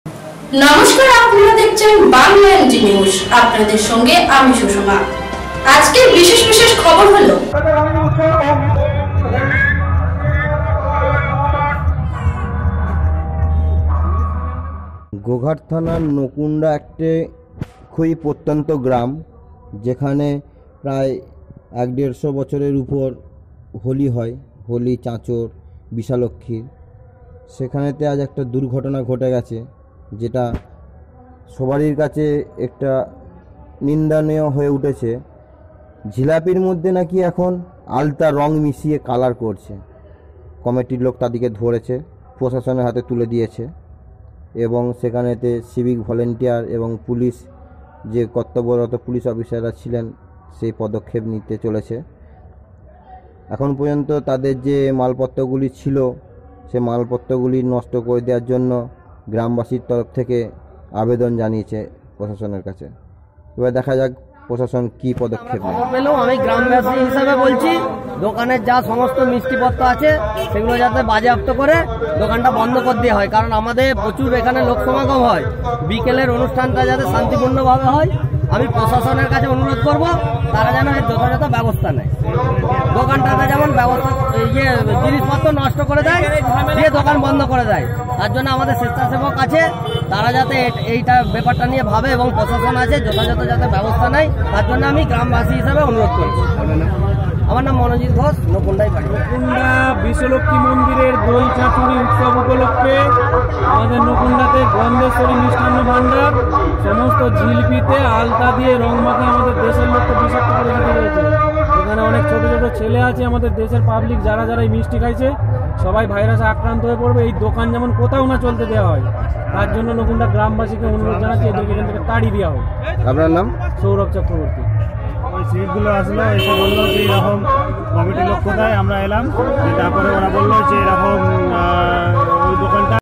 মিষ্টি সঙ্গে আলতা র রং মেশানো হচ্ছে, এমনই ঘটনা ঘটলো গোঘাটের নকুণ্ডা গ্রামে। ঘটনাস্থলে পুলিশ।
সত্যের সন্ধান
এগিয়ে থাকি.